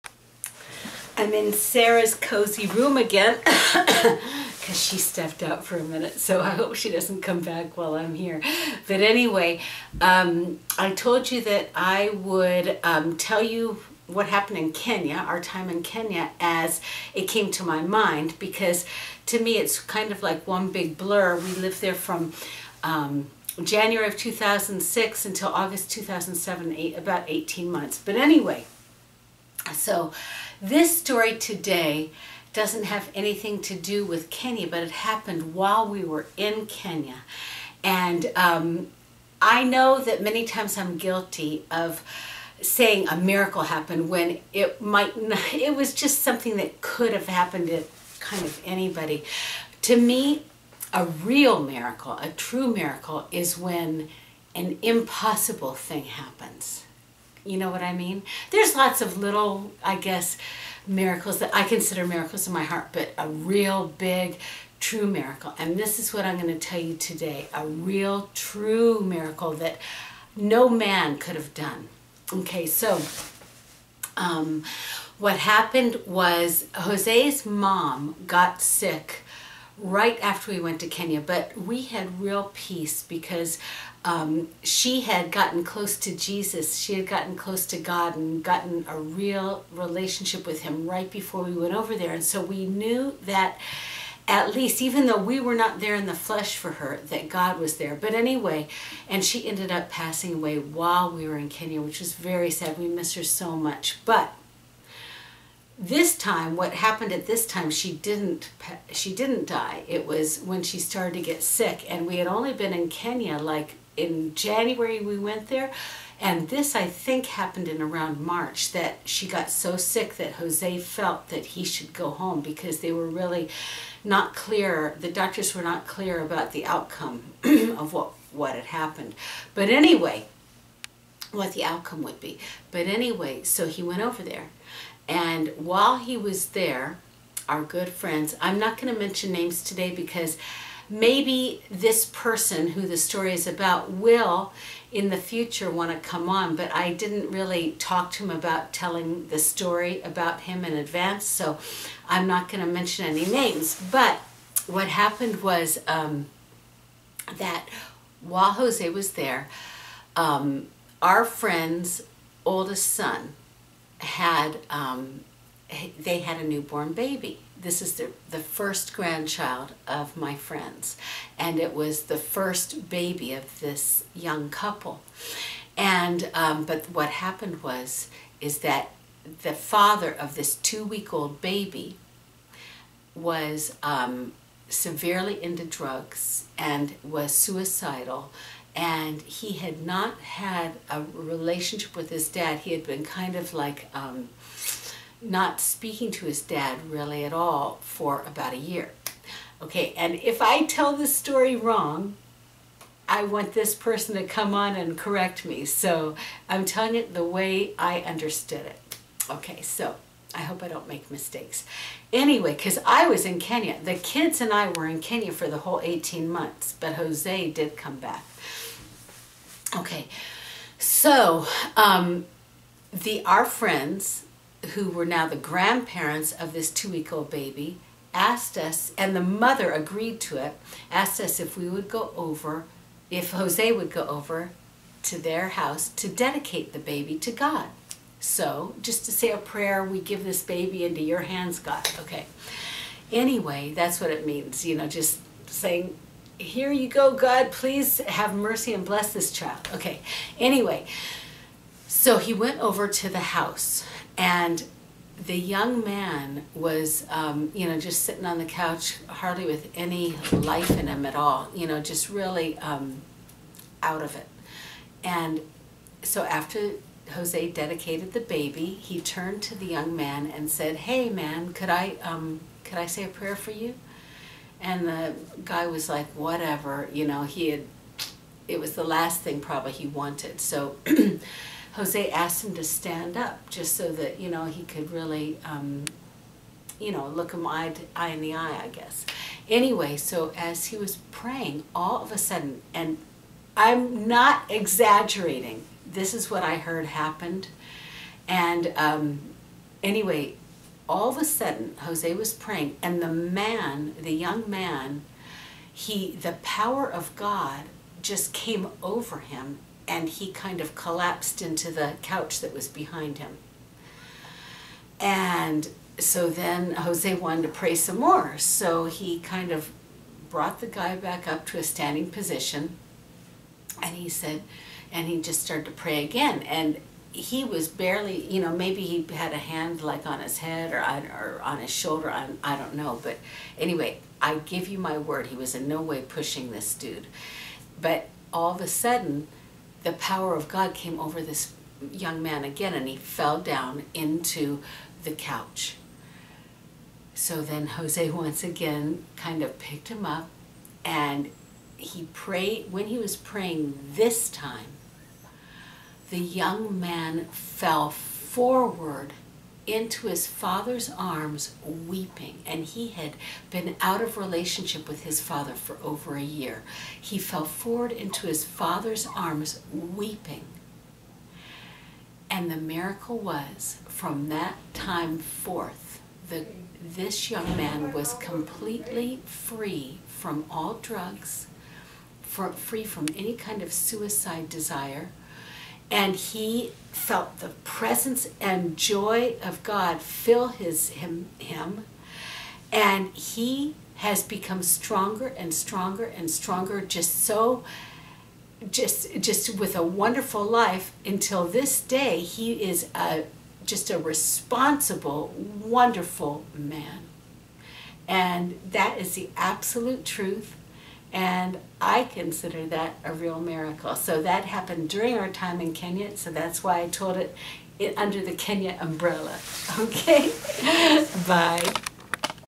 A True Miracle
A story of instant freedom from drugs and suicide